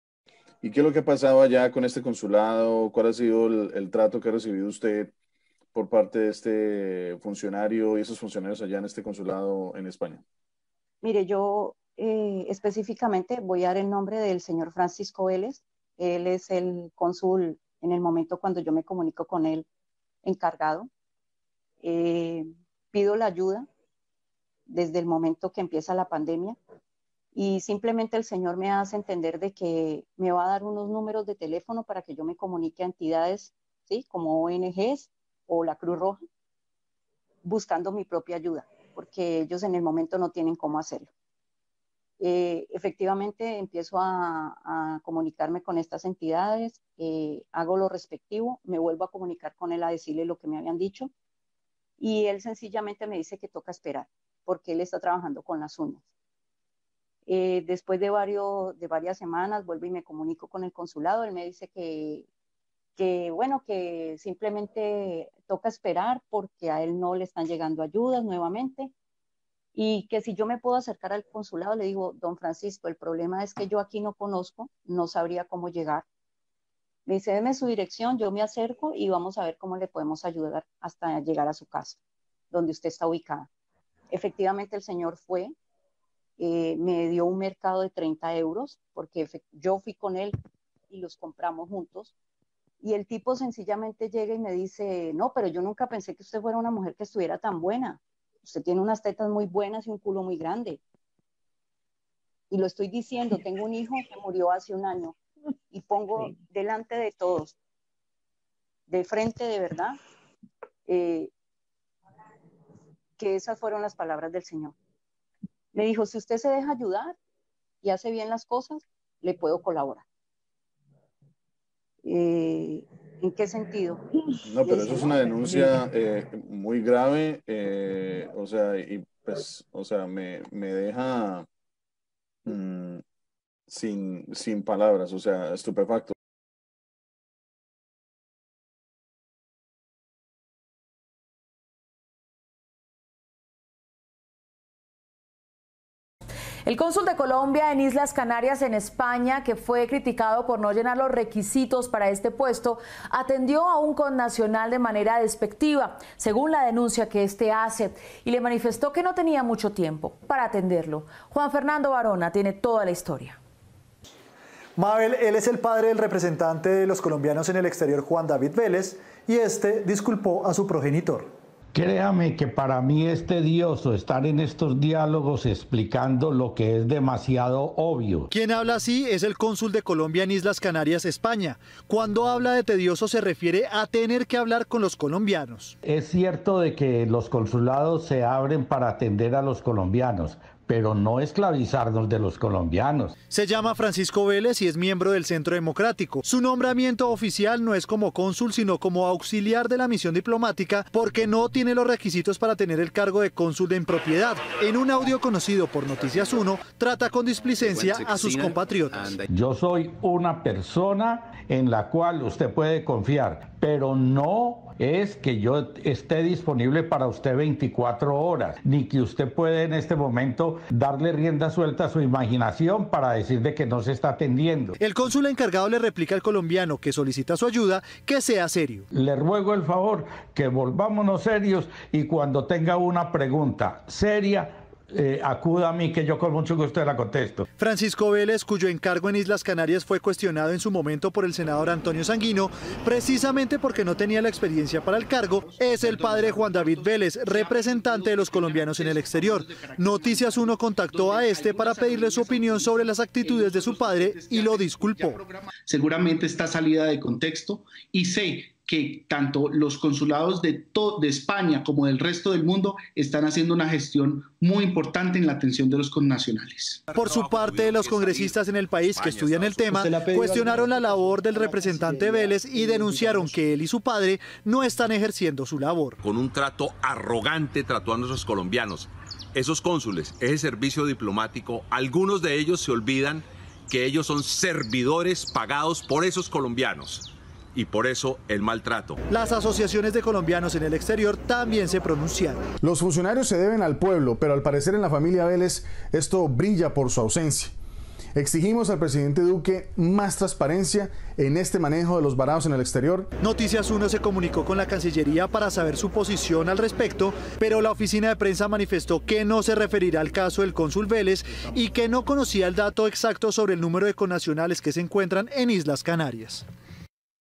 🔴 Consul Uribista acosador sexual
El Cónsul (que en realidad es auxiliar de misión diplomática):
- No reúne los requisitos para el cargo
- Es acosador sexual de la Colombiana Erika Maria Restrepo Vitali
- Es vago
- Es Uribista
- Es el Padre de Juan David Velez @JuanDaVelez, representante a la Cámara por Colombianos en el exterior del @CeDemocratico